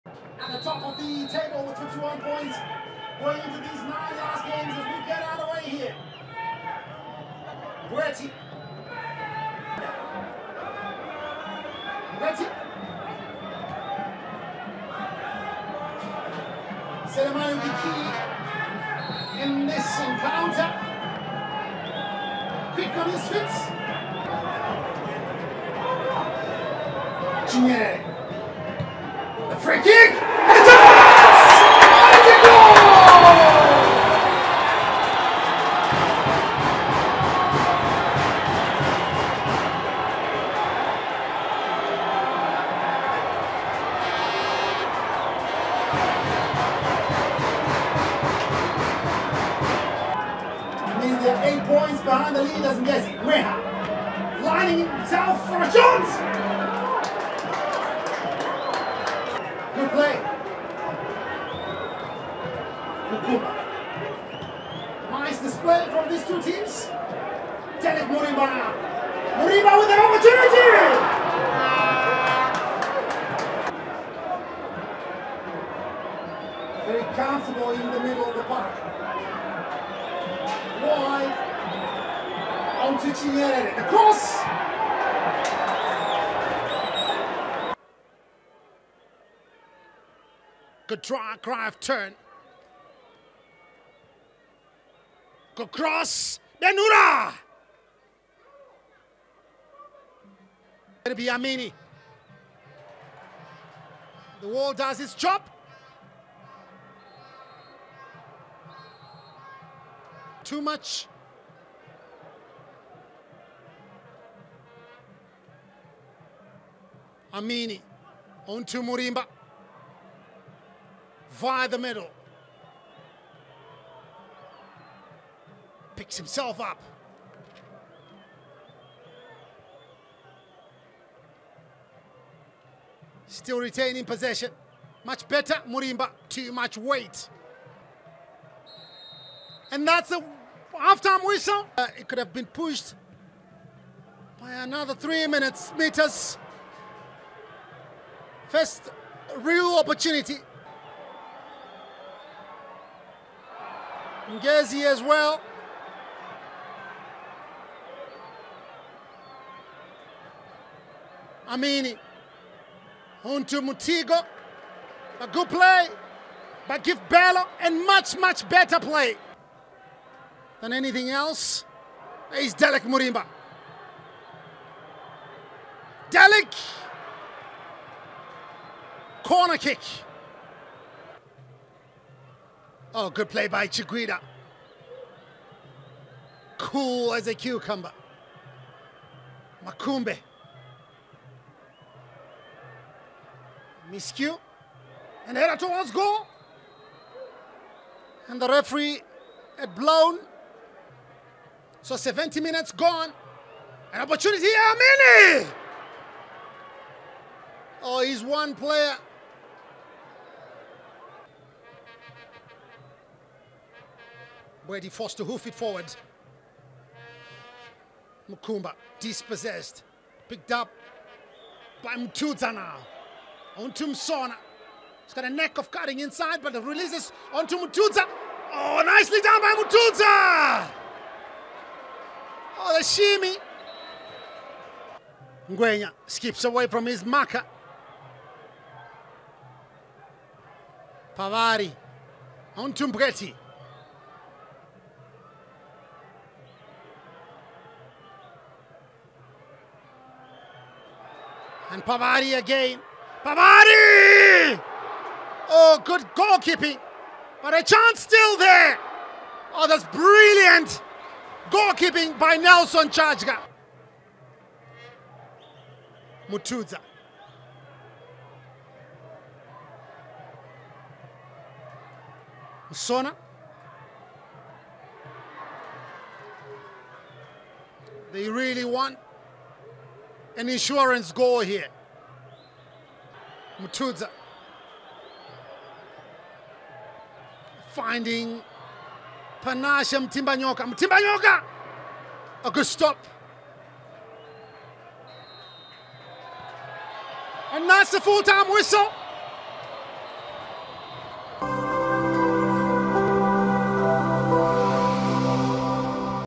HIGHLIGHTS | FC Platinum vs Ngezi Platinum Stars | Platinum Derby Delivers | ZTN Prime | MD26
FC Platinum vs Ngezi Platinum Stars highlights for the match played at Mandava Stadium.